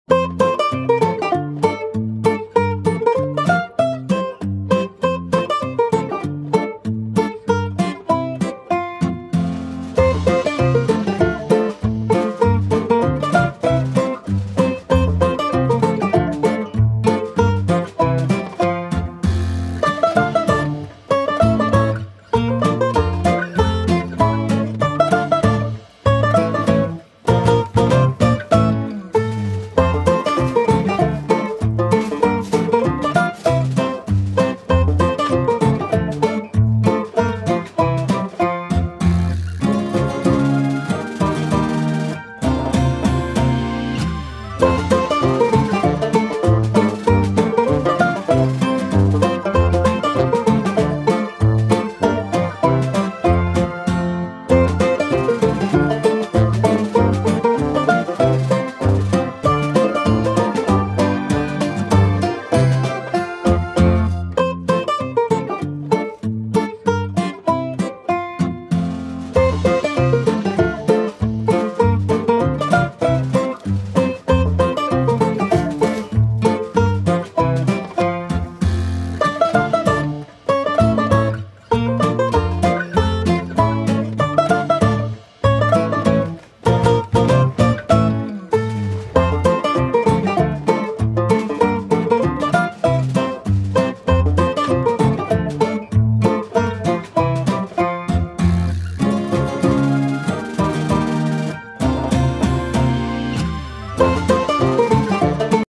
When the lettuce is bigger than you are!
Patient cats endure rodent and human antics at breakfast time